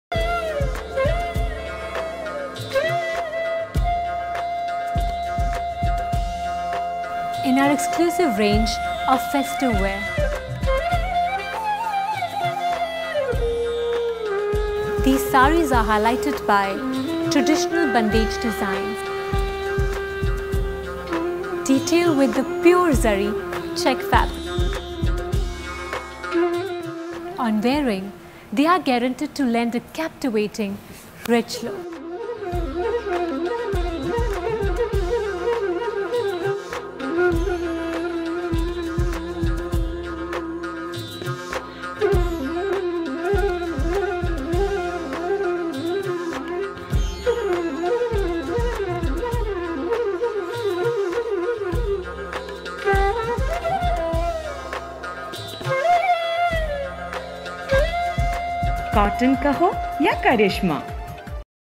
Mangnee Pattu by Karishma Sarees
These sarees are enhanced by traditional bandhej designs enriched with pure zari. It Is one of the special festive wear from our collection.
This collection will provide you with a captivating rich look.

To locate the closest dealer in your region, please visit our